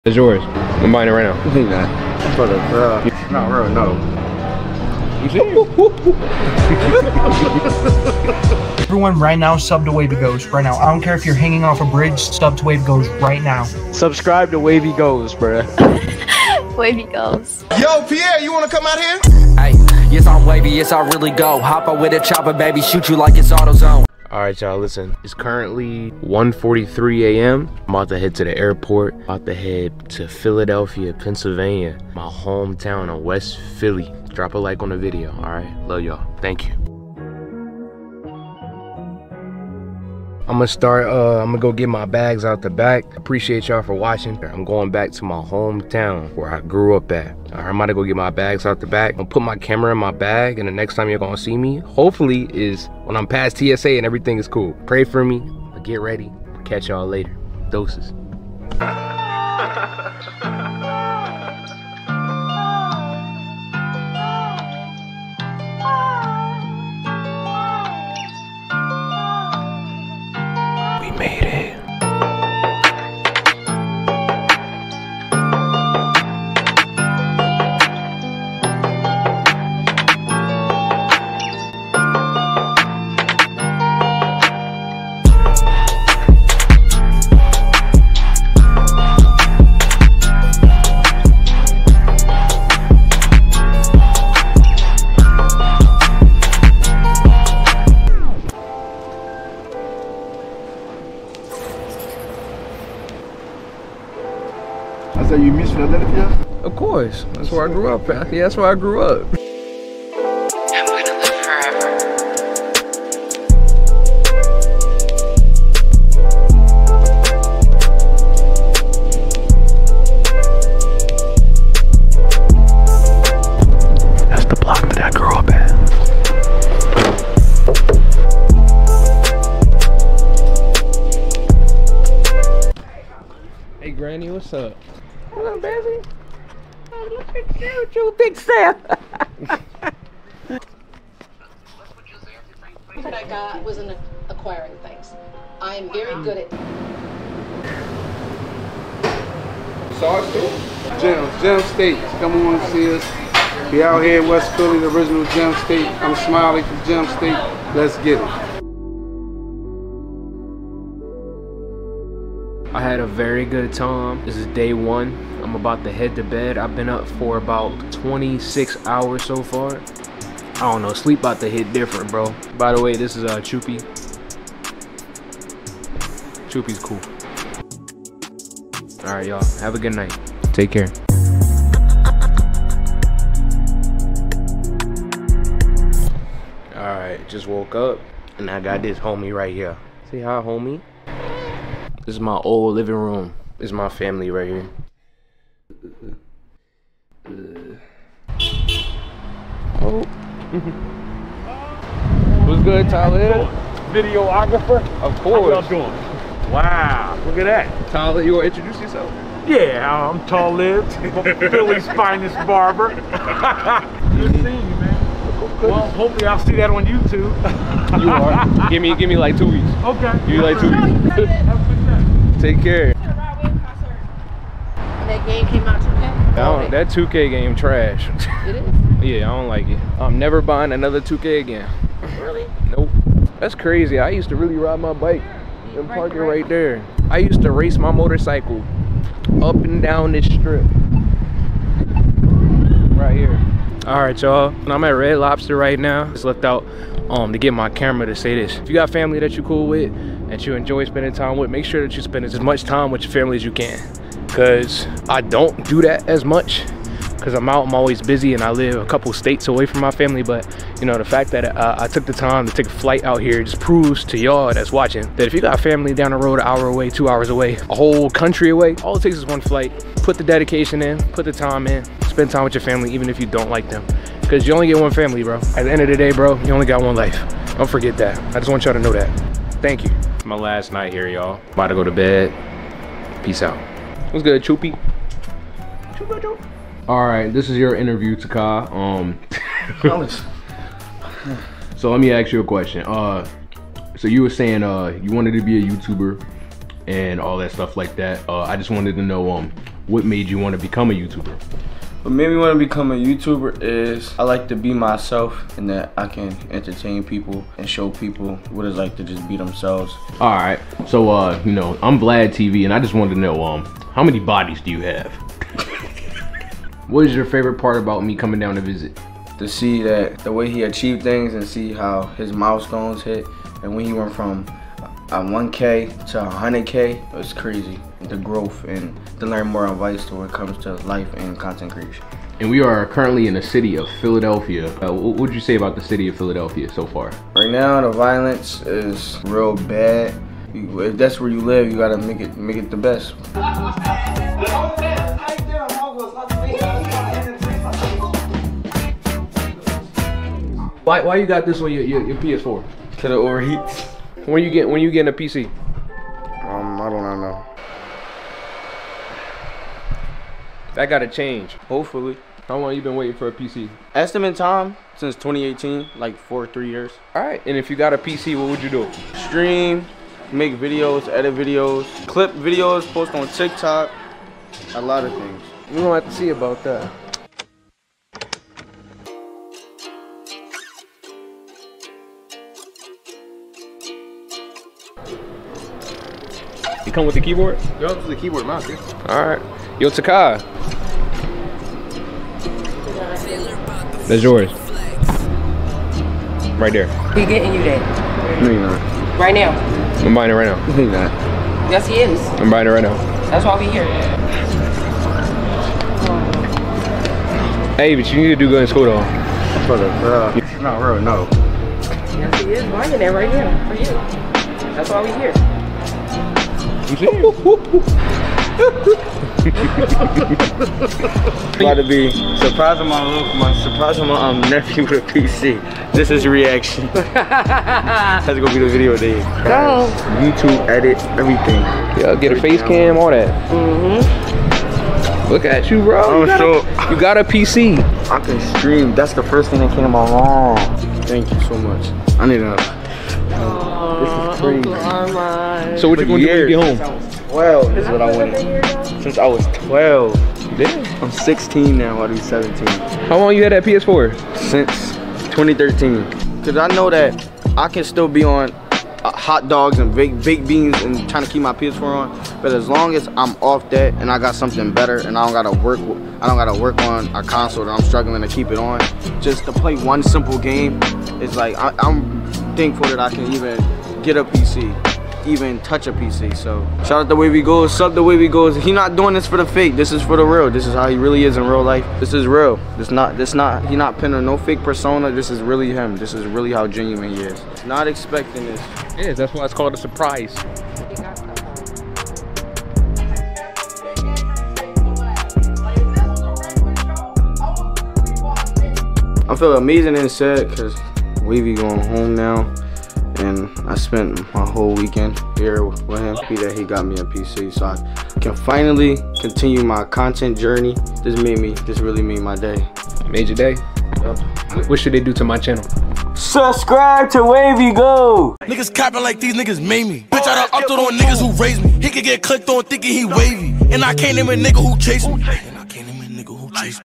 BOUGHT HIS FIRST GAMING PC EVER!! | WEST PHILLY HOOD VLOG!!
Hope you guys enjoyed this video. Let me know if you liked the video or not so I can improve on my creative content :)